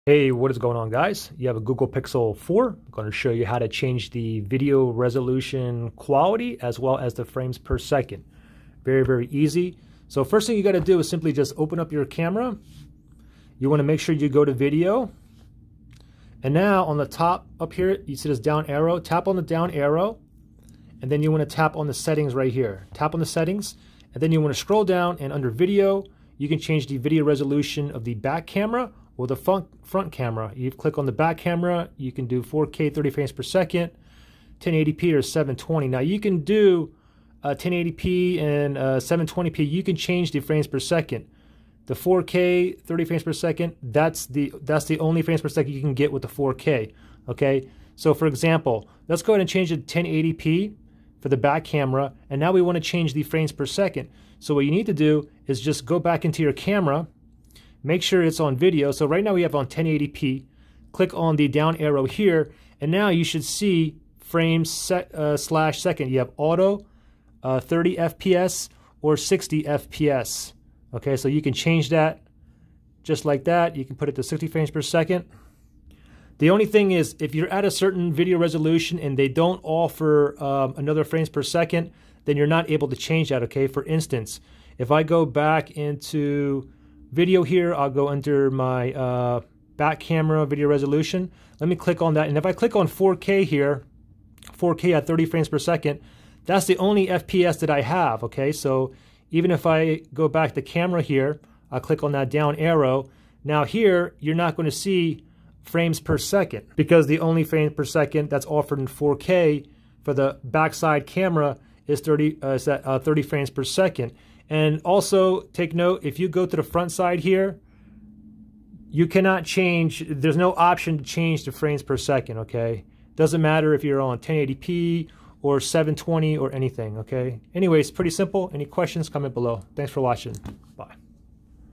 Google Pixel 4 / 4XL: How to Change Video Resolution Quality & FPS (Frames Per Second)
I show how to change the video resolution quality and fps on the Google Pixel 4 and 4XL. Hope this helps.